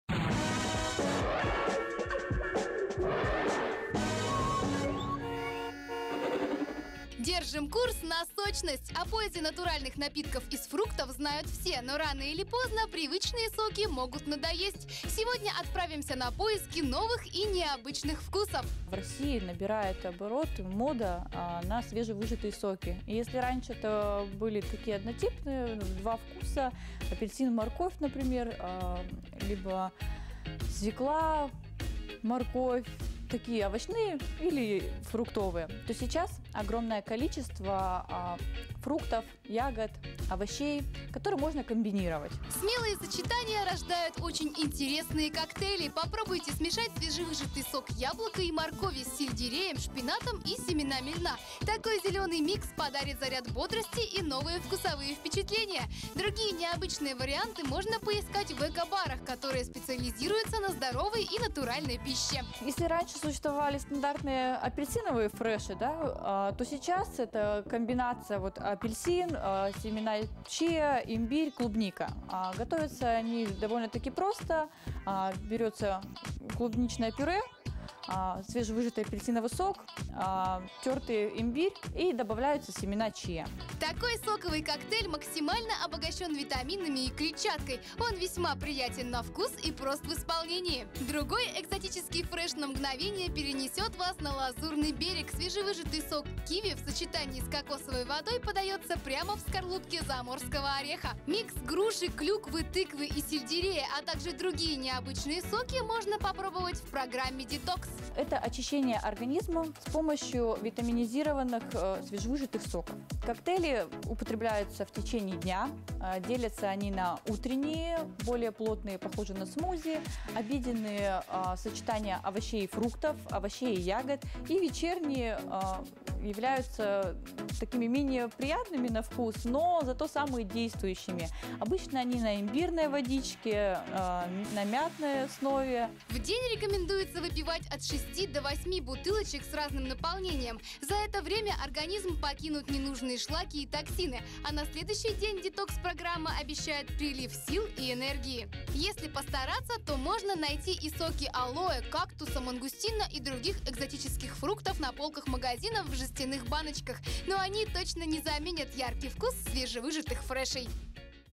В самом соку!
Сделайте выжимку - из фактов, из овощей, фруктов и ягод. Такое задание мы выдали гиду. Удалось ли ему отыскать необычные фреши и нектары - скоро узнаем.